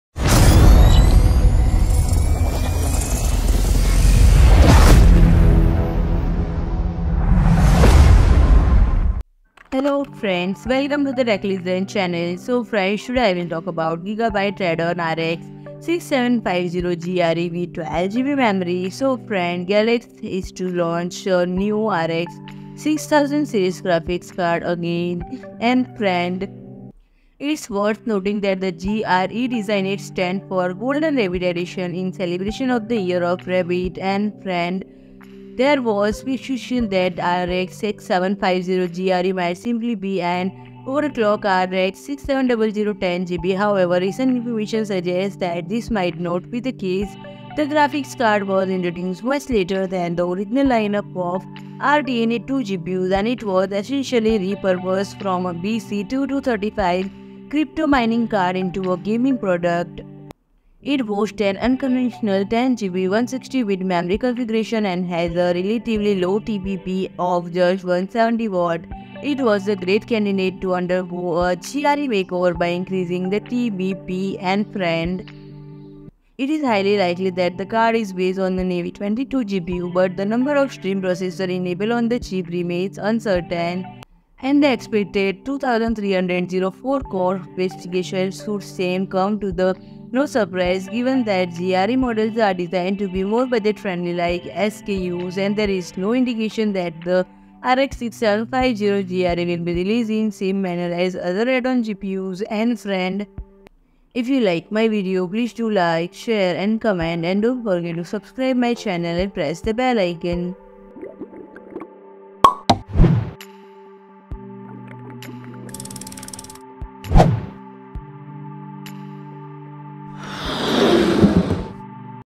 Gigabyte Radeon RX 6750 GRE 12GB Graphics Card Launch Soon - Explained All Spec, Features And More
PRODUCT BRAND STORE AMAZON

there have been ongoing rumors circulating on the internet for the past two months about a potential release of the RX 6750 GRE graphics card. This model is expected to follow the debut of the RX 7900 GRE, which was introduced in late July. It’s worth noting that the “GRE” designation stands for “Golden Rabbit Edition,” in celebration of the Year of the Rabbit in China. Consequently, this card is not available for purchase through the DIY market outside of China, but it can be obtained through various system integrators globally. This graphics card was introduced much later than the original lineup of RDNA2 GPUs, as it was essentially repurposed from a BC-2235 cryptomining card into a gaming product. It boasts an unconventional 10GB 160-bit memory configuration and has a relatively low TBP (board power) of just 170W. It was a great candidate to undergo a “GRE makeover” by increasing the TBP.